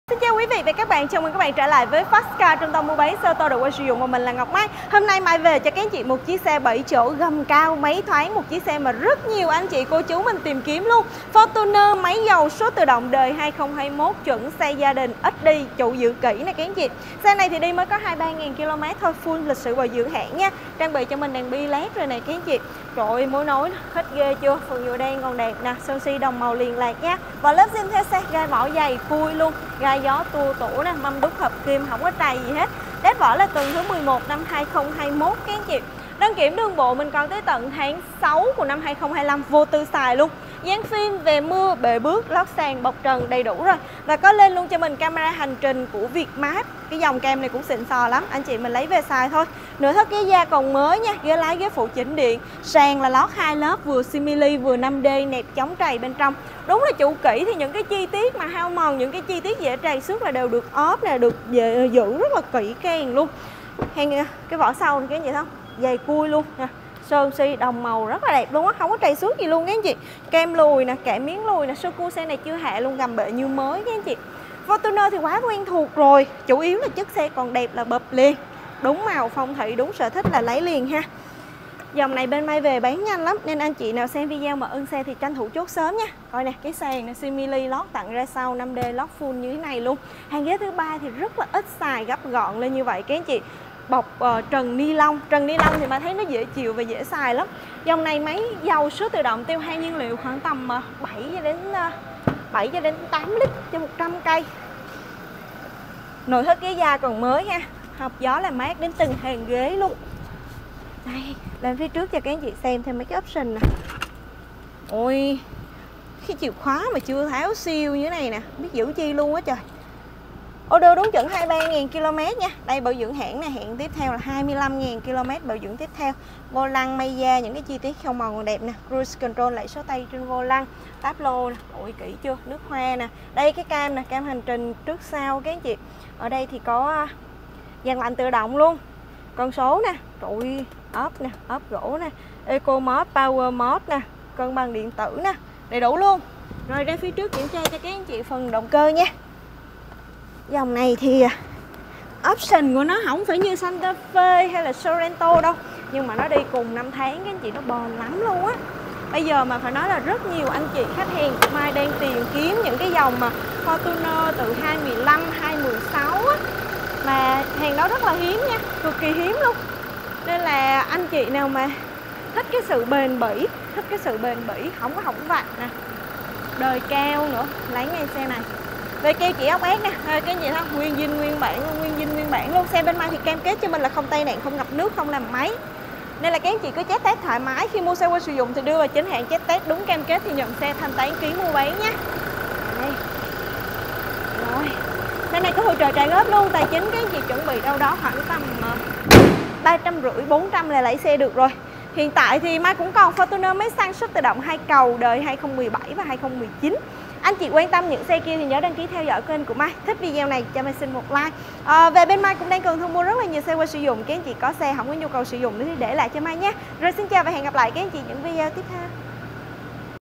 Xe 7 chỗ máy dầu Fortuner 2021 2.4AT chuẩn xe gia đình ít đi 23.000km full lịch sử hãng | 0917914O44
Hot🔥 Fortuner chuẩn xe gia đình, ít đi
🚘 Fortuner 2.4AT 2021 máy dầu số tự động
- Odo 23.000 km, bảo dưỡng hãng đủ
- Vỏ zin theo xe
- đăng kiểm, đường bộ đến t6/2025

- Đã dán film, bệ bước, camera lùi, cảm biến lùi...

- Trang bị: ghế chỉnh điện, cruise control, lẫy chuyển số vô lăng, cân bằng điện tử, màn hình cảm ứng, gương chiếu hậu tích hợp cam hành trình, cam & cảm biến lùi, lót sàn 5D...

- Giá 9xx tr - Trả trước 350 tr nhận xe